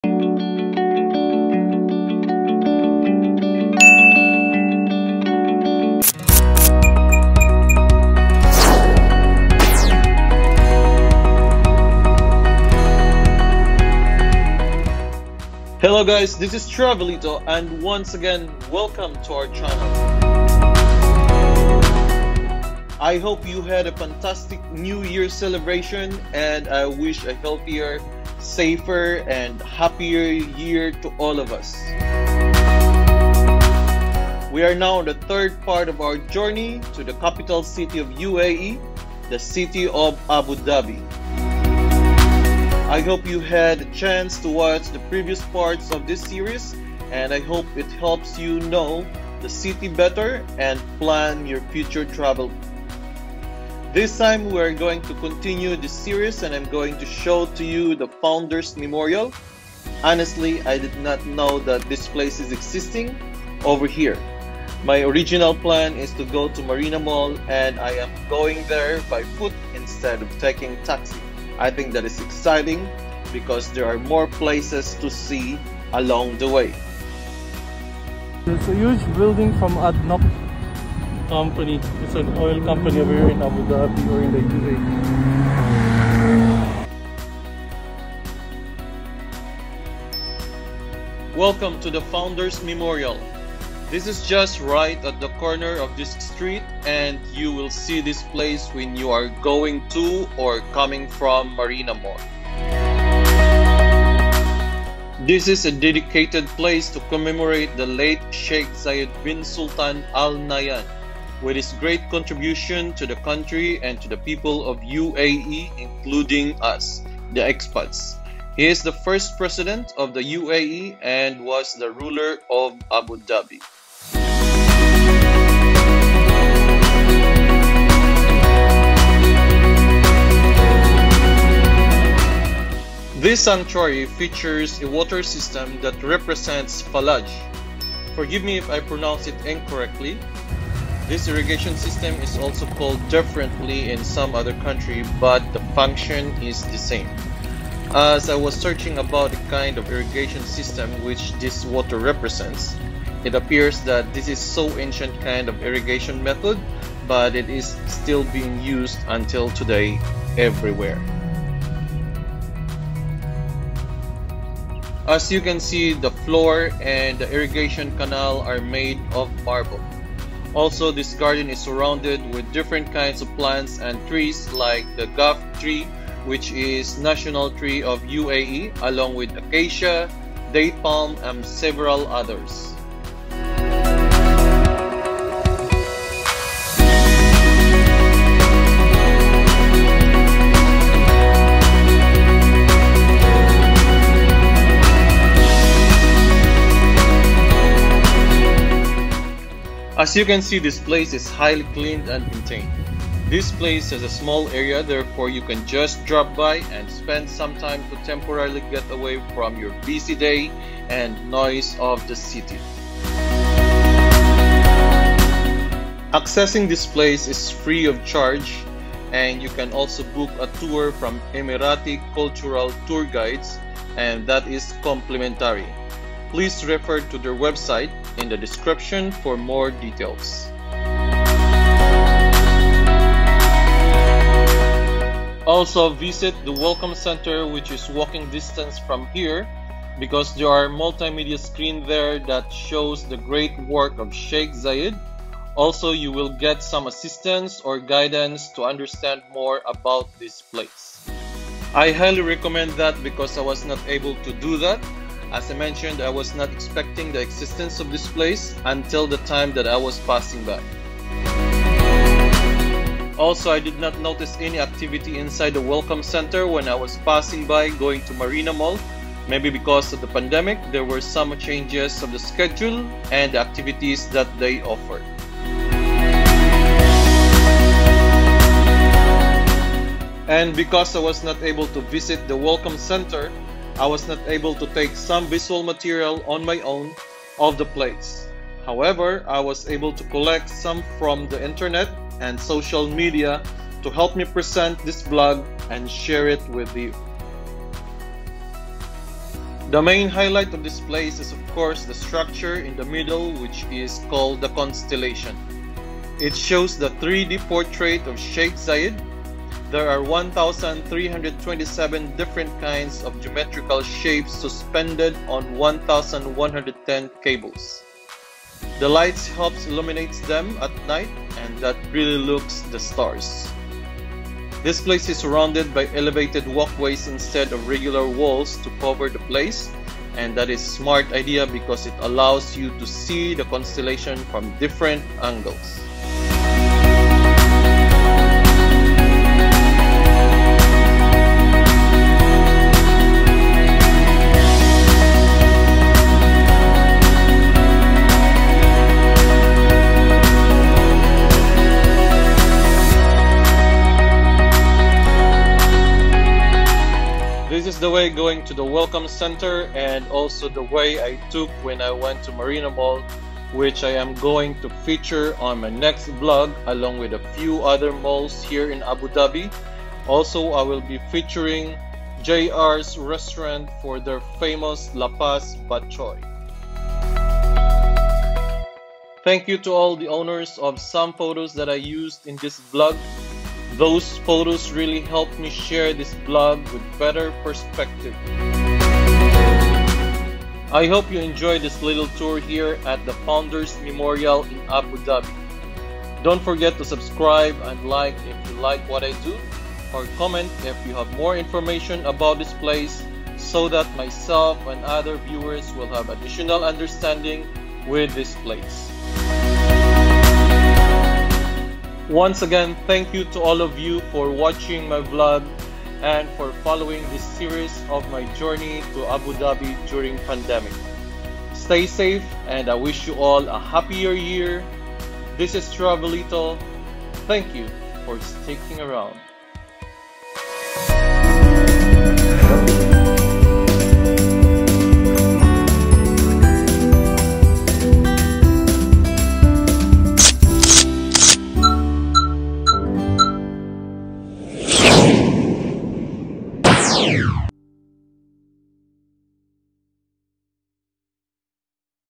Abu Dhabi tour| The Founder's Memorial | Sheikh Zayed Bin Sultan Al Nahyan
Unexpected visit in The Founder's Memorial.

The original plan was to go to Marina mall. But while walking, I saw this place and I was so curious. I immediately went up and I saw the water irrigation and the constellation in the middle.

I did not know that time what it was and it's significance. But the place is beautiful and peaceful.

Some photos in this vlog was taken from internet and social media to be able to effectively show to you the beauty of this place.